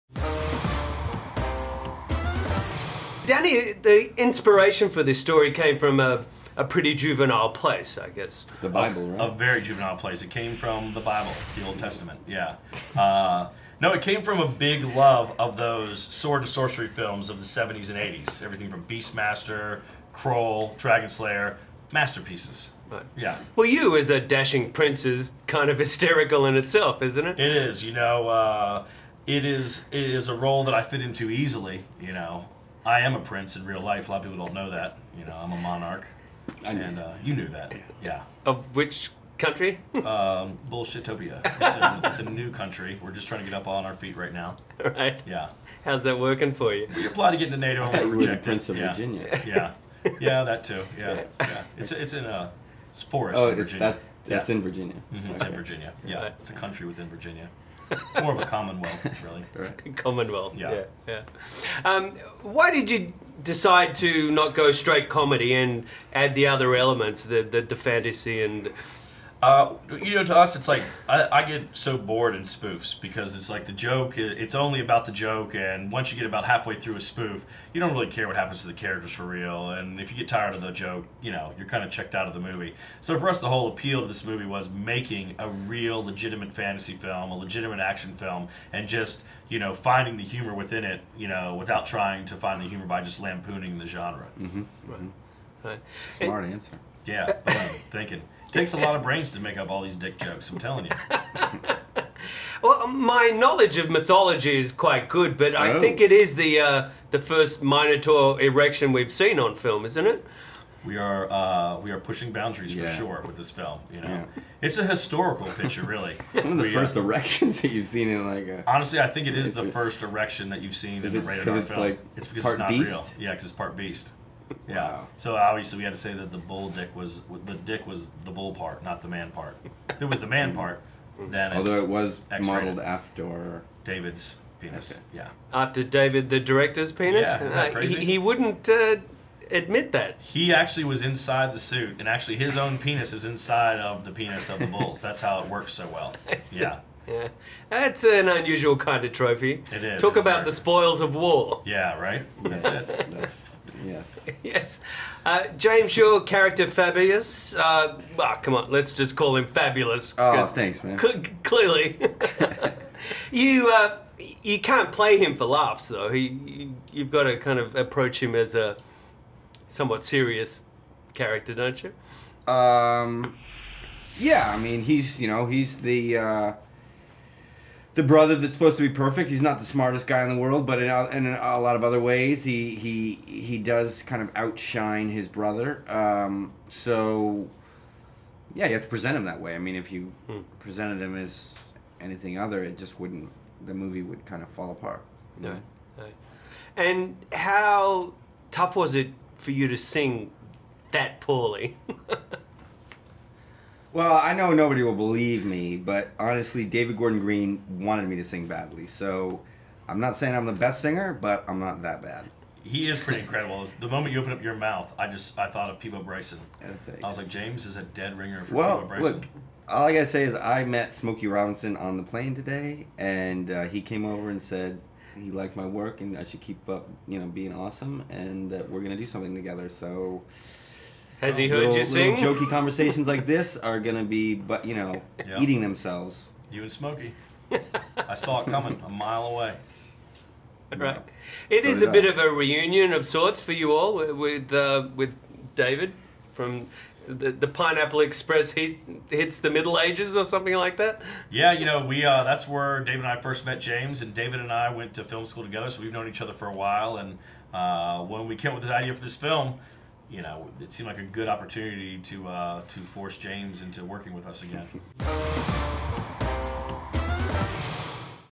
STAR Movies VIP Access: Your Highness - James Franco & Danny McBride
STAR Movies interviews James Franco and Danny McBride of Your Highness.

Become a fan of VIP Access on Facebook now for exclusive giveaways, breaking Hollywood news and the latest trailers!